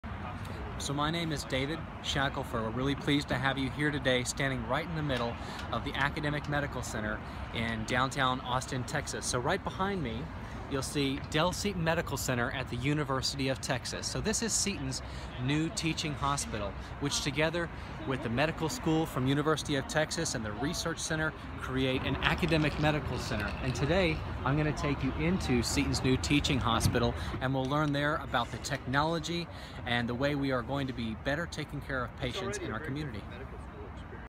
TOUR the new Dell Seton Medical Center at the University of Texas
Take a look inside the new teaching hospital that officially opens to the public in May 2017.